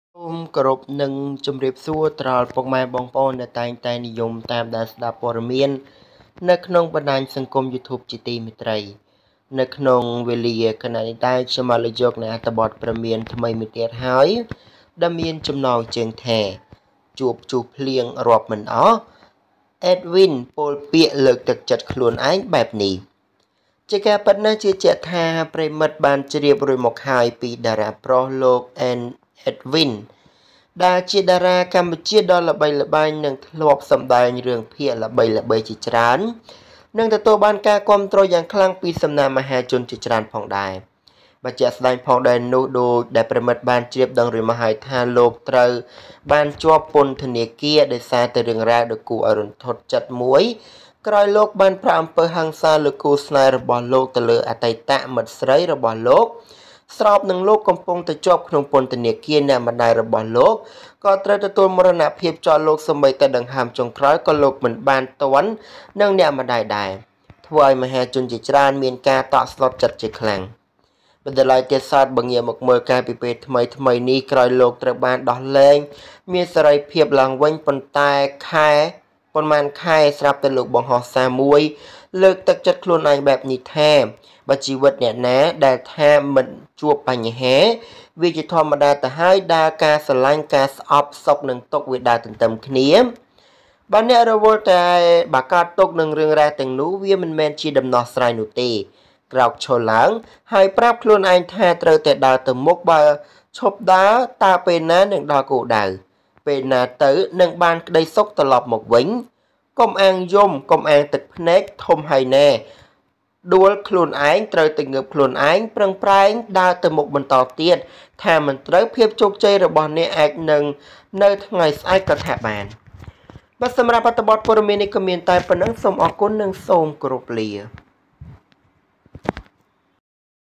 ជួបព្យុះភ្លៀងមករាប់មិនអស់ អេដវីន ពោលពាក្យលើកទឹកចិត្តខ្លួនឯងបែបនេះ
In the face of countless rainstorms, Edwin said words of encouragement to himself.

@Sombon kh @

@Sombon kh @